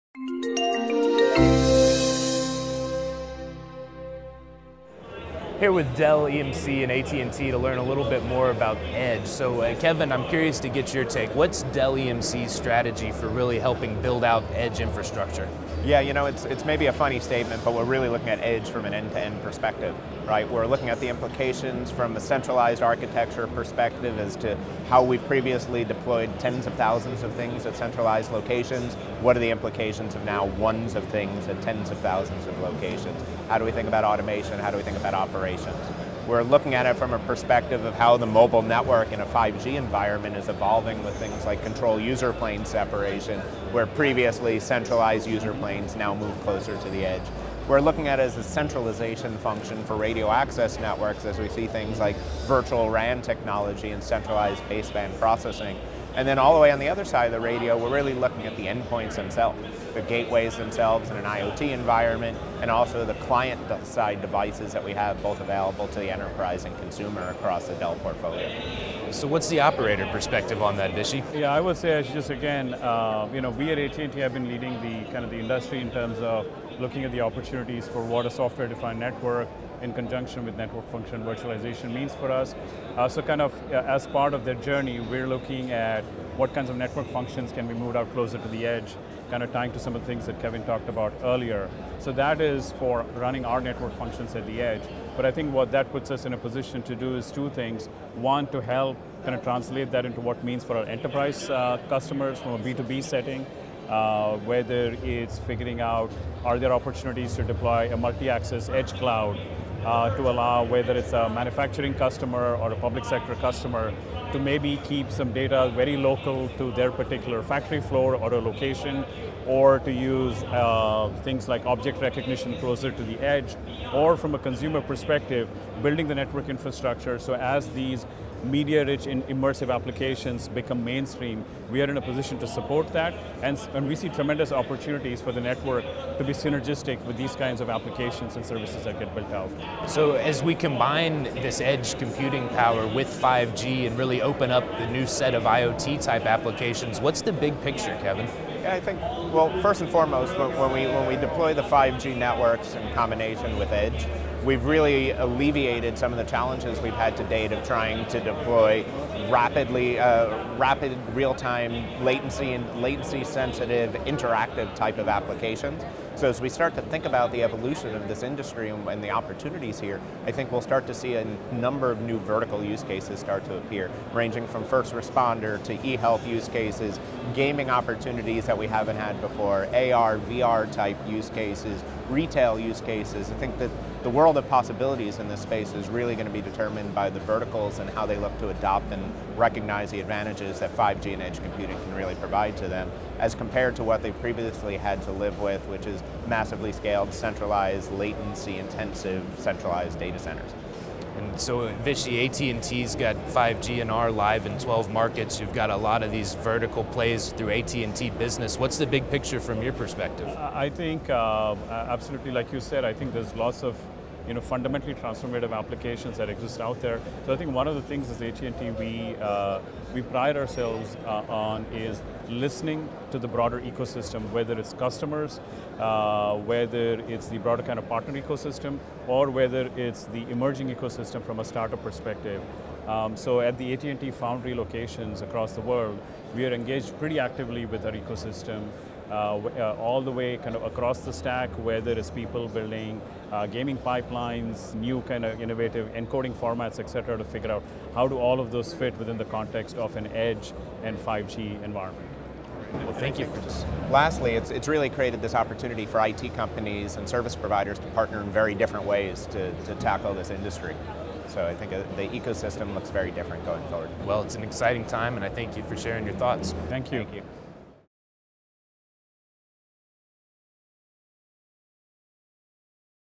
Living on the Edge: An Interview with Vishy Gopalakrishnan and Kevin Shatzkamer
Vishy Gopalakrishnan, Vice President, Ecosystem & Innovation - Wireless Technology, AT&T and Kevin Shatzkamer Senior Vice President Networking and Solutions, Dell EMC discuss the state of innovation at the edge